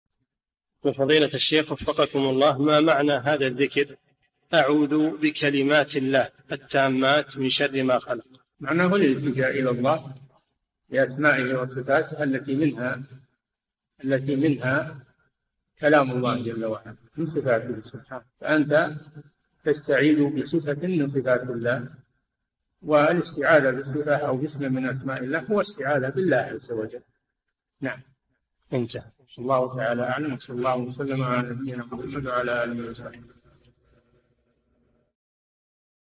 [1390- 4717] معنى هذا الذكر:(أعوذ بكلمات الله التامات من شر ما خلق) - الشيخ صالح الفوزان
[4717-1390] ما معنى هذا الذكر: ((أعوذ بكلمات الله التامات من شر ما خلق))؟
الجَوَابُ: معناه الالتجاء إلى الله بأسمائه وصفاته التي منها كلام الله جل وعلا ومن صفاته سبحانه، فأنت تستعيذ بصفة من صفات الله والاستعاذة بالصفة أو باسم من أسماء الله هو استعاذة بالله عز وجل.
والله تعالى أعلم، وصلى الله وسلم على نبينا محمد وعلى آله وصحبه.
السيرة الذاتية :
للاشتراك في قناة الشيخ صالح الفوزان
لمشاهدة حلقات فتاوى على الهواء كاملة

للاستفسار والإقتراحات والدعم
رئيس الفريق
لمشروع كبار العلماء